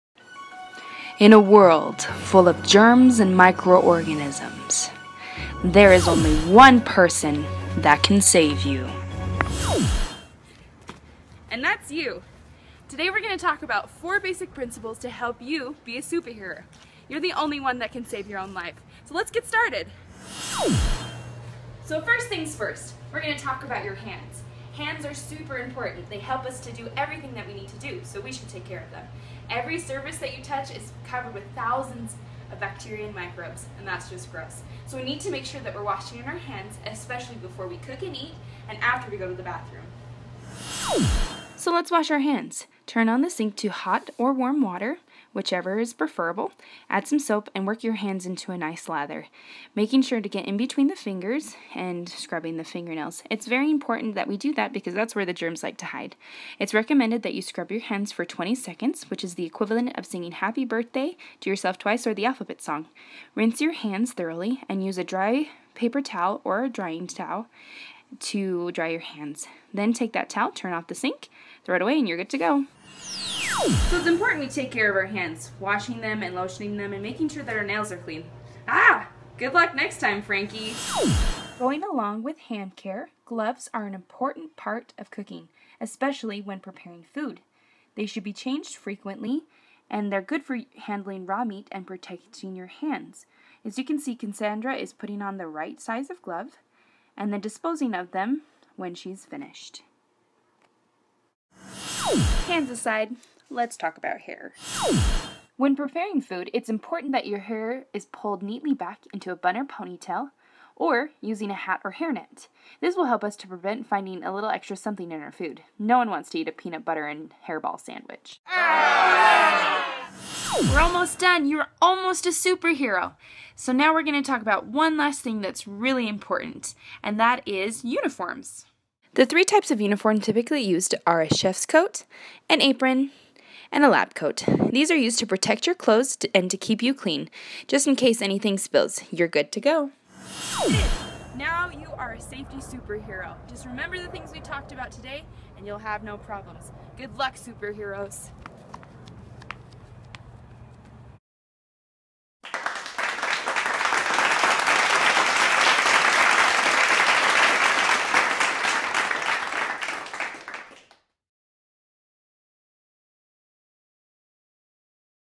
Super Safety
An educational movie produced to teach the UEN core Principle of hygiene.  In this movie you will learn how to properly wash there hands, use gloves, proper hair restraint, and use of uniforms.